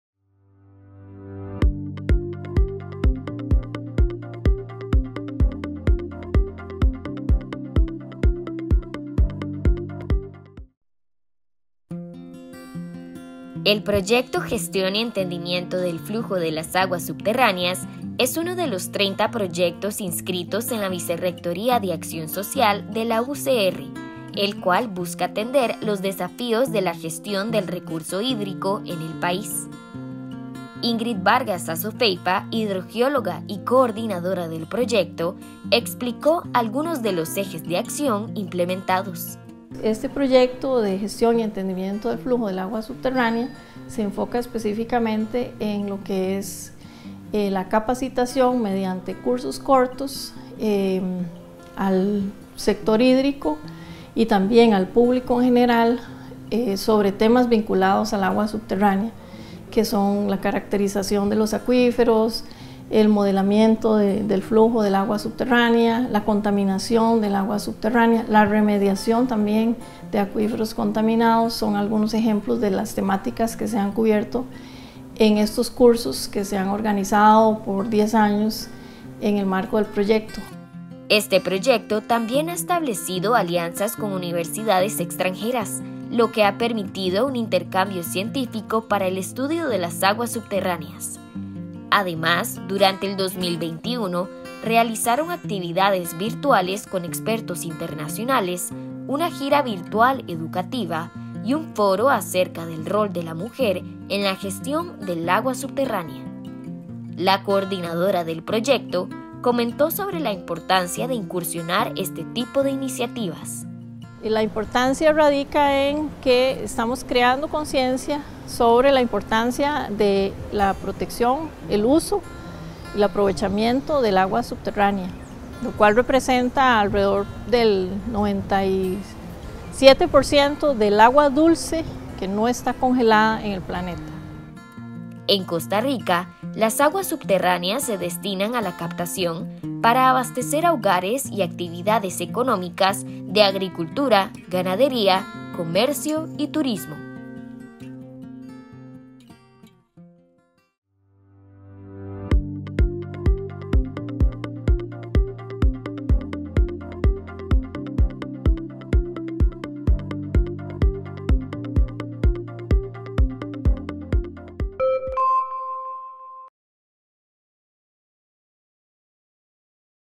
Resguardo y control de la calidad de las aguas subterráneas en Costa Rica
Conozca uno de los 30 proyectos de acción social que buscan educar e incidir en el manejo sostenible del agua.